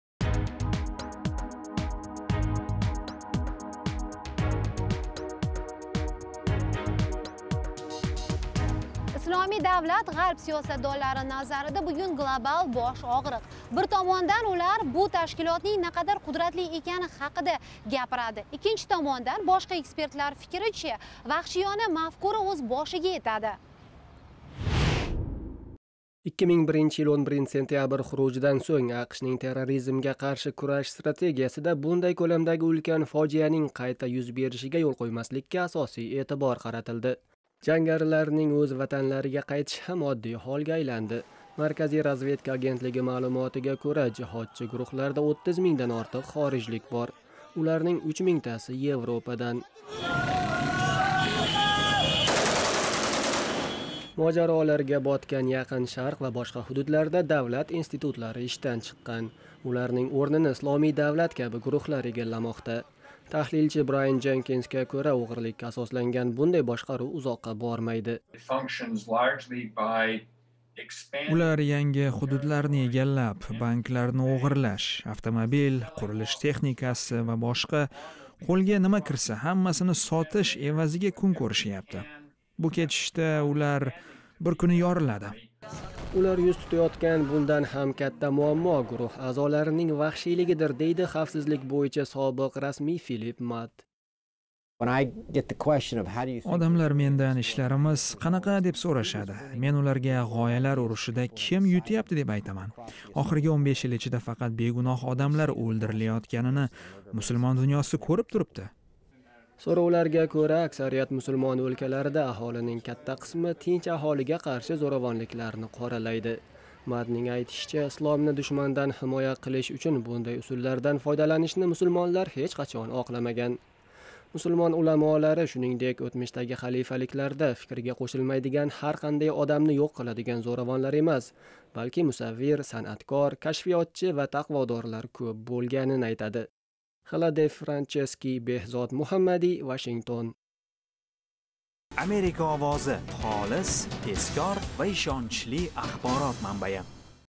"Islomiy davlat" bilan kurash
Ko'plab ekspertlar nazarida zamonaviy terrorizm turli ko'rinishlarda shu darajada tez rivojlanyaptiki, terrorizmga qarshi bugungi chora-tadbirlar ularga bas kela olmayapti. Boshqa bir guruh kuzatuvchilar fikriga ko'ra esa, terrorchilar bunday vahshiy mafkuralari bilan uzoqqa bormaydi.